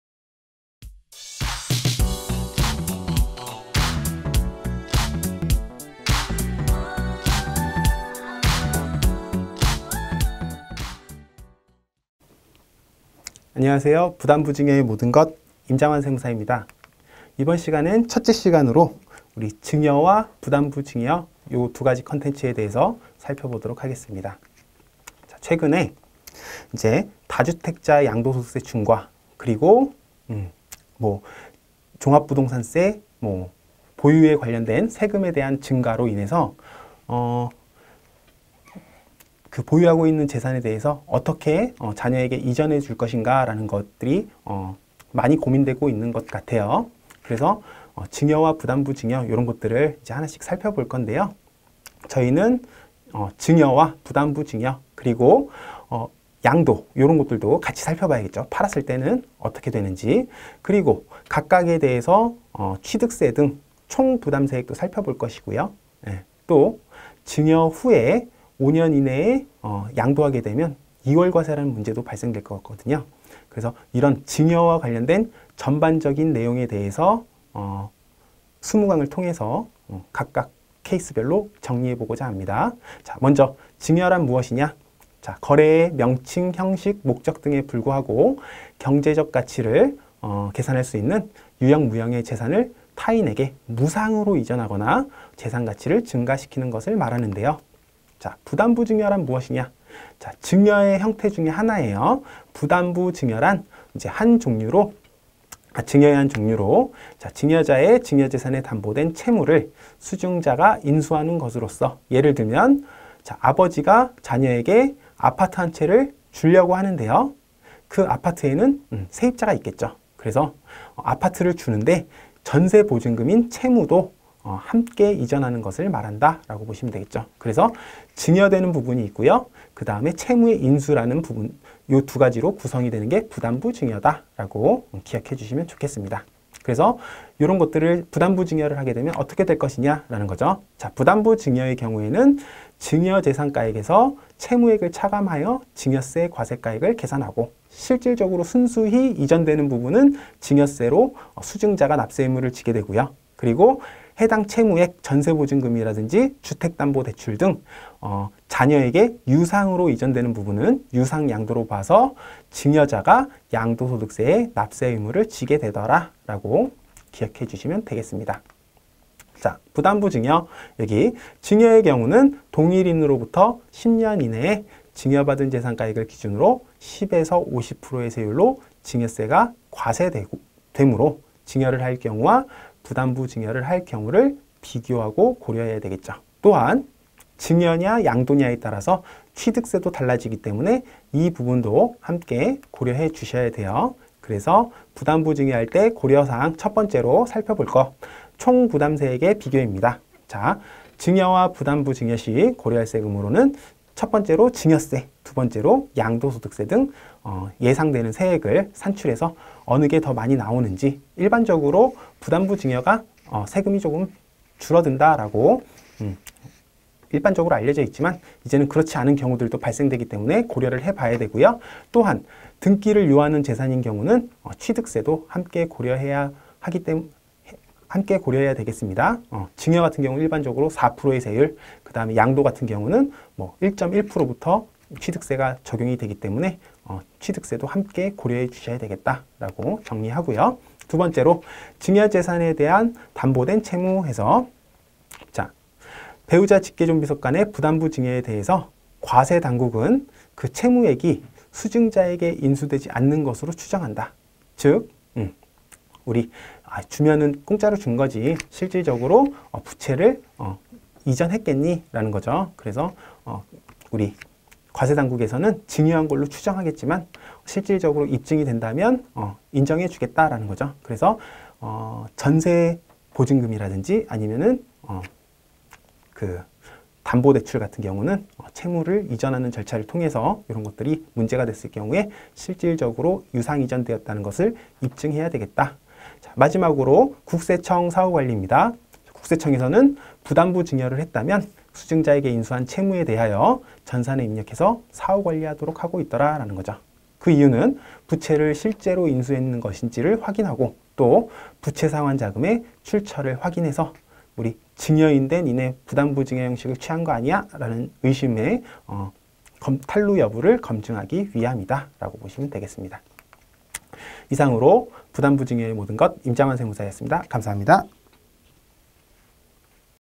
부담부증여의 모든 것, 증여 VS 부담부증여 - 임장환세무사의 5분특강 #1
1. 대(大)제목 : 부담부증여의 모든것
2. 소(小)제목 : 증여 VS 부담부증여
3. 소소(小小)제목(3가지) :
      1) 증여
      2) 부담부증여
      3) 부담부증여시 고려사항